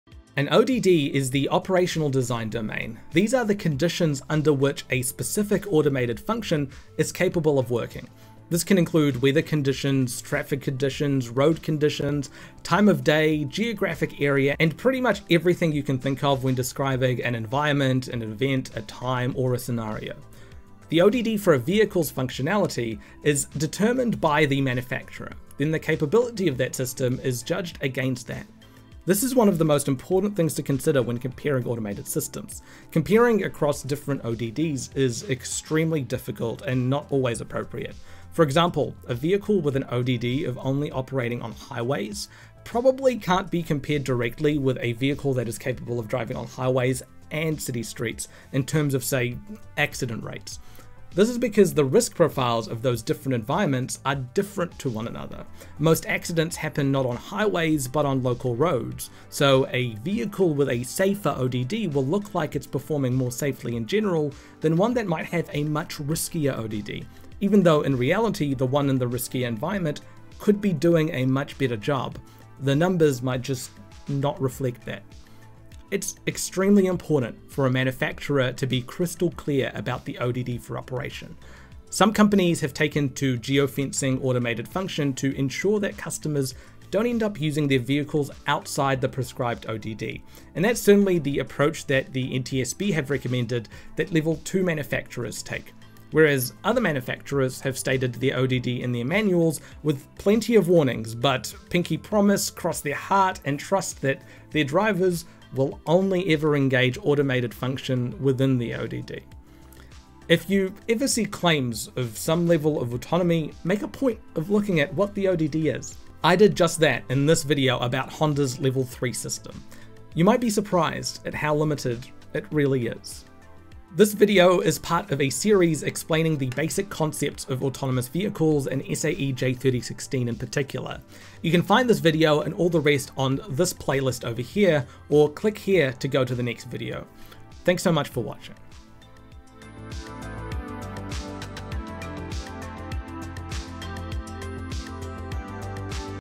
What is the Operational Design Domain (ODD)? | Autonomous Vehicles 101
The Operational Design Domain (or ODD) is the measuring stick we use to determine how effective a self driving car or autonomous vehicle is. But what can be part of a driverless car ODD? Who determines it and can we compare between different ODD's?

This video quickly covers what the Operational Design Domain is from an SAEJ3016 context